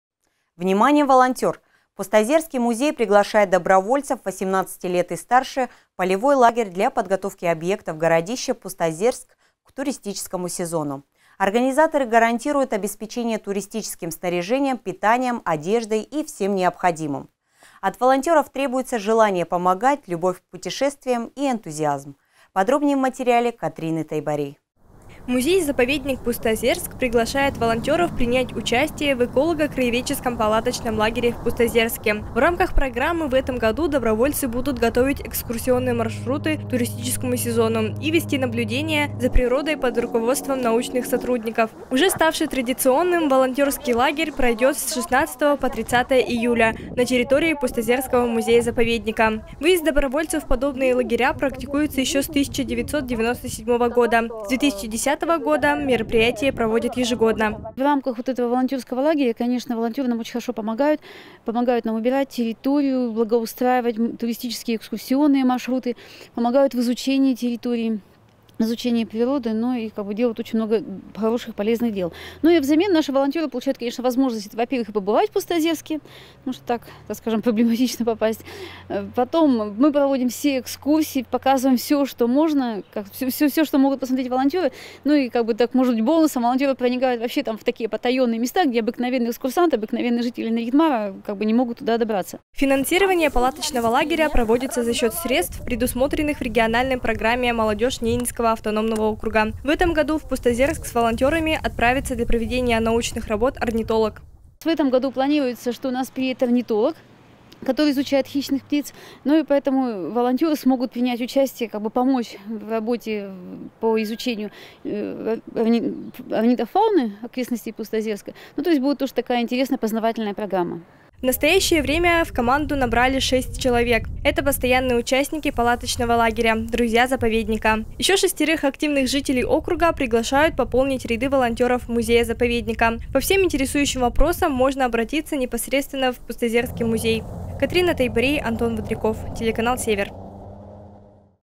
ОТДЫХ В ПУСТОЗЕРСКЕ С ПОЛЬЗОЙ
Внимание, волонтер! Пустозерский музей приглашает добровольцев восемнадцати лет и старше в полевой лагерь для подготовки объектов городища Пустозерск к туристическому сезону. Организаторы гарантируют обеспечение туристическим снаряжением, питанием, одеждой и всем необходимым. От волонтеров требуется желание помогать, любовь к путешествиям и энтузиазм.